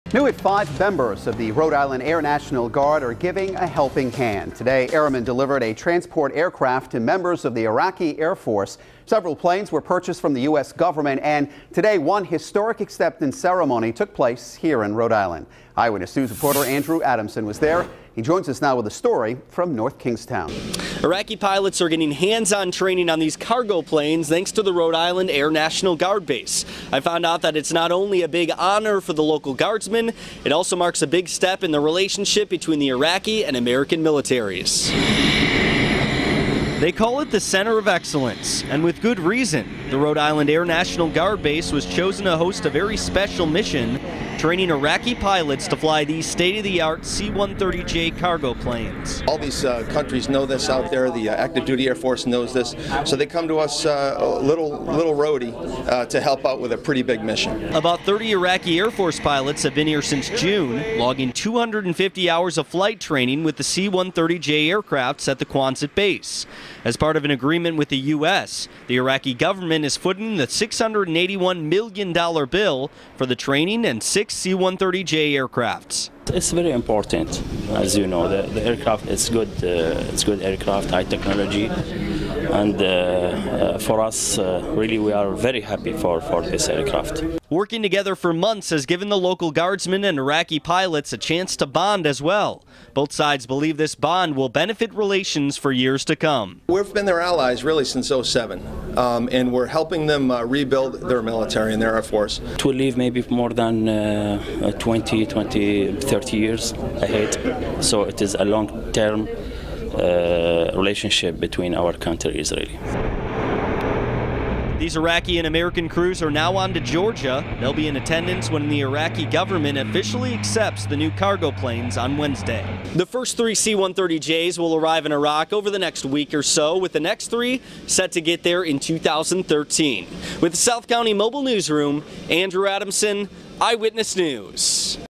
RI Guardsmen to Train Iraqi Pilots
Airmen from the RI Air National Guard held a ceremony to present a transport aircraft to members of the Iraqi Air Force.